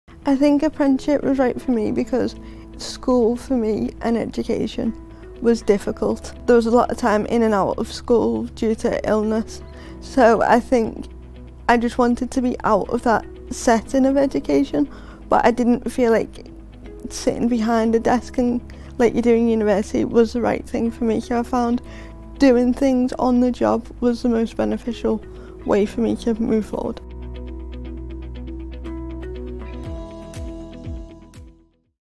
Abbie (Short 1) - Liverpool City Region Combined Authority
Following the launch of the new Be More Career Portal, Abbie from the Liverpool City Region Combined Authority, gives us first hand insight into how an apprenticeship has helped kick start her career, and how the new career portal can transform the careers of others too.